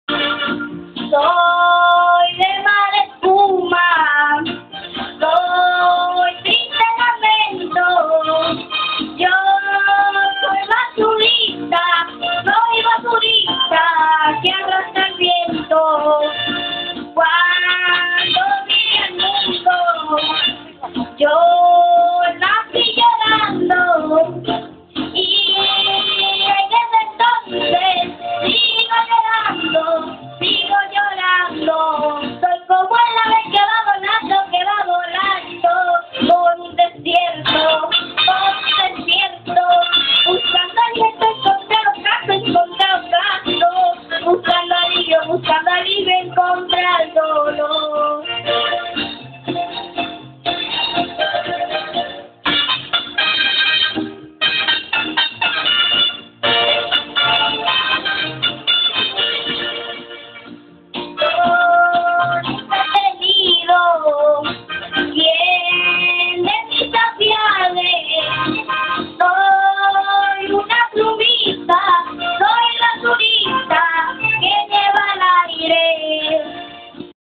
la rancherita de bruzual
la niña paola urdaneta iniciando sus dotes artisticos en la musica cantando en el genero de la ranchera vive en clarines estado anzoategui venezuela